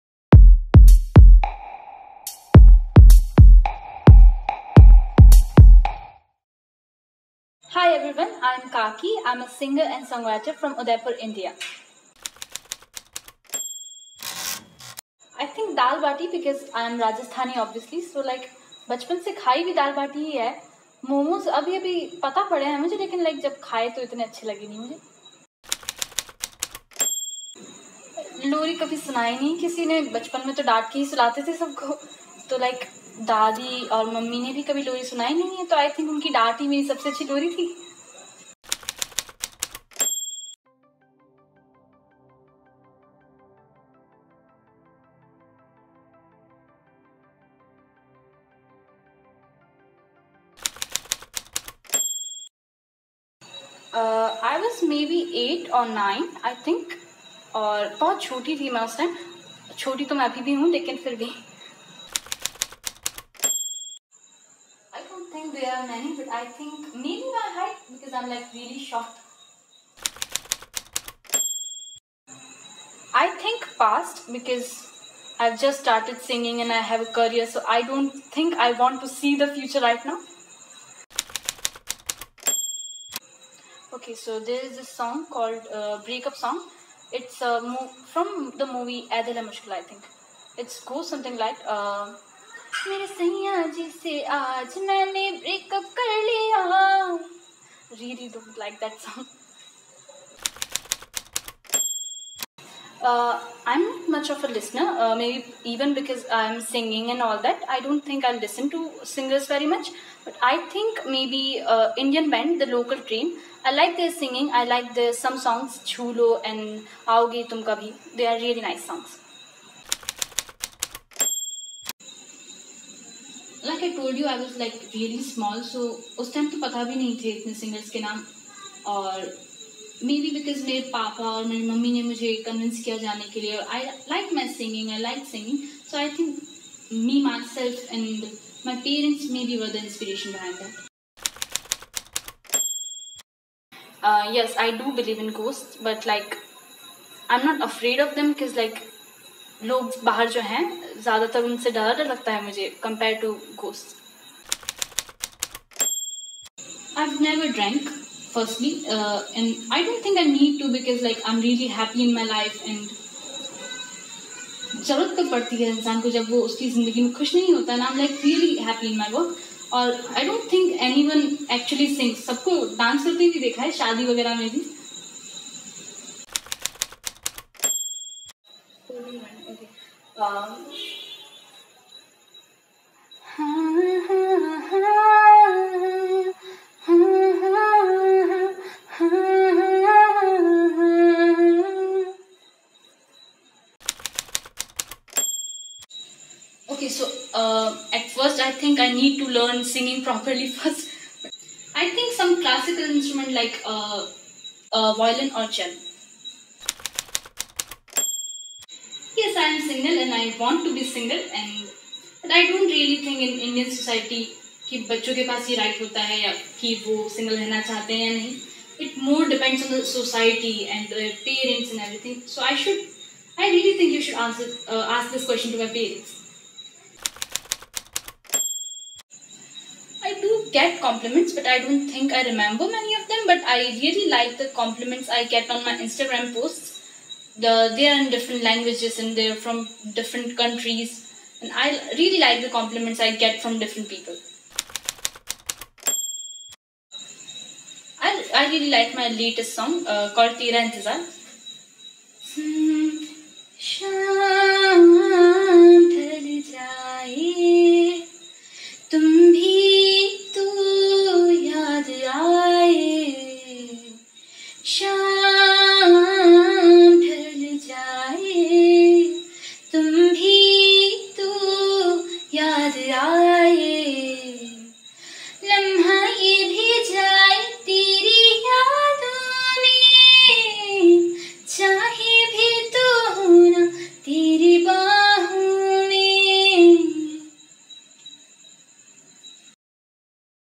Getting To Know KAKI SINGER | EP - 1 | Talentsofworld ( 9xm.tv ) Chat with Kaki Singer
Kaki Joins conversation with Talentsofworld ( 9xm.tv ) and disclosed some of her Personal Views, Likes, and Dislikes.

Kaki (casually known as Hema Dangi) is from Udaipur, India. Kaki in her underlying music accounts is seen singing English tunes. She has moreover sung some English-Indian old-style blends.  She took her first step in the music world at the age of nine and has been learning and practicing various styles of music from Indian Classical to Rap/Hip-Hop to Electronic Music.